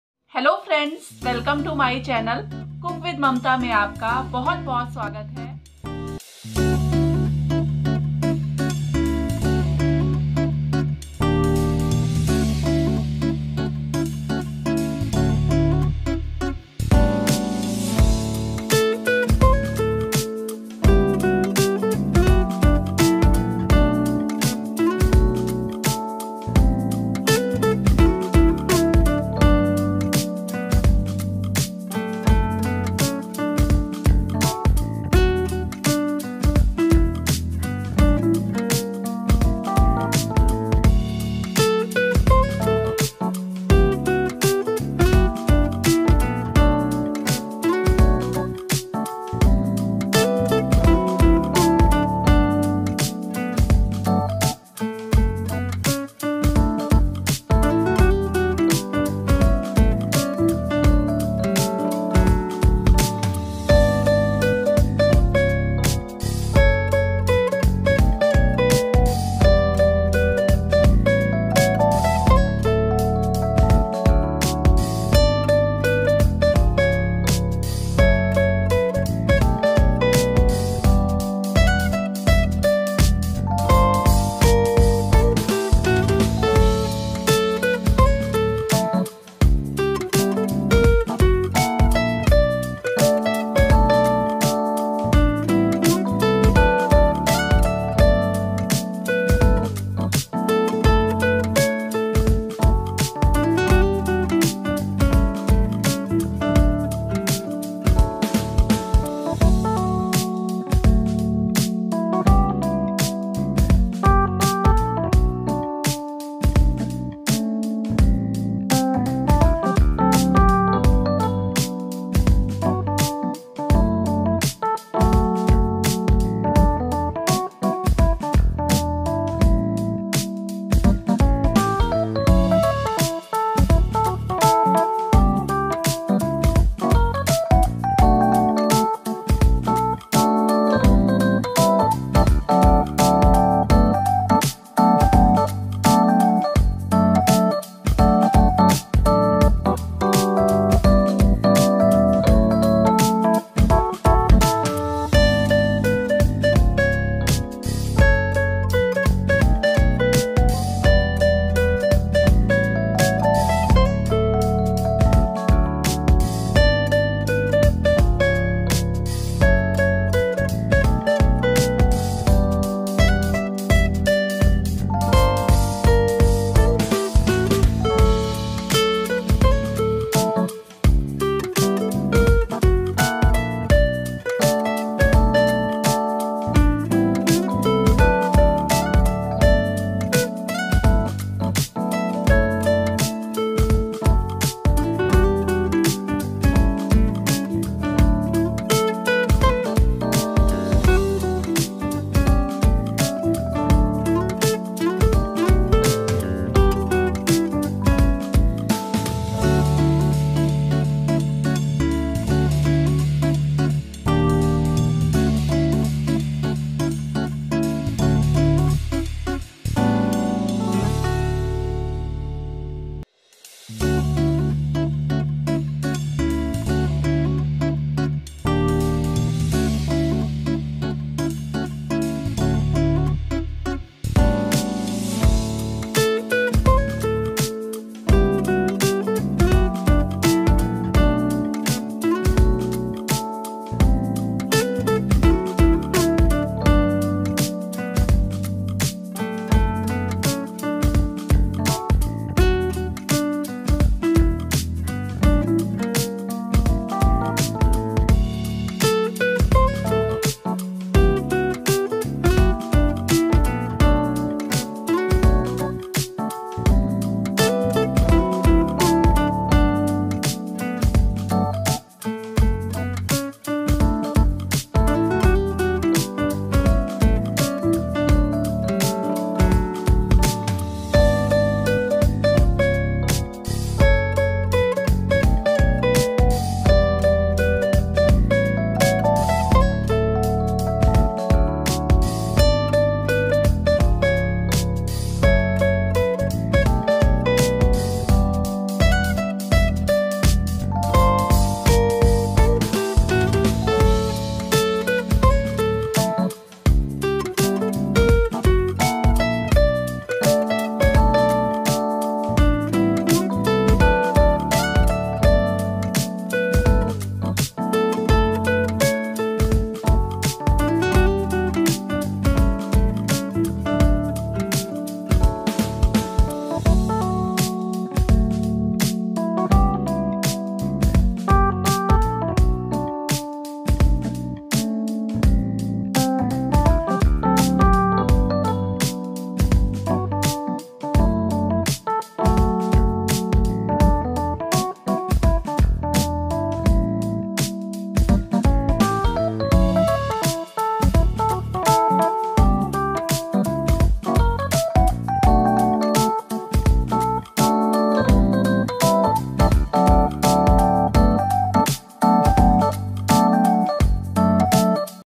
दिवाली पर बनाए बिना आग जलाऐ दो अलग तरह की मिठाईयां और पाए सब का प्यार
दिवाली पर बनाए बिना आग जलाऐ दो अलग तरह की मिठाईयां और पाए सब का प्यार ‎@Cook With Mamta

Instant mawa apple Barfi
Instant sattu laddu
Apple Barfi banane ka Aasan tarika
Sattu ke laddu

lngredients for mawa apple Barfi
200 gram mava
200 gram desiccated coconut
250 gram Boora or powder sugar
2 tbsp milk
1 pinch red food colour

Ingredients for sattu laddu
1 cup chana sattu
3/4 Cup boora or powder sugar
1/2 cup ghee
1 pinch yellow food colour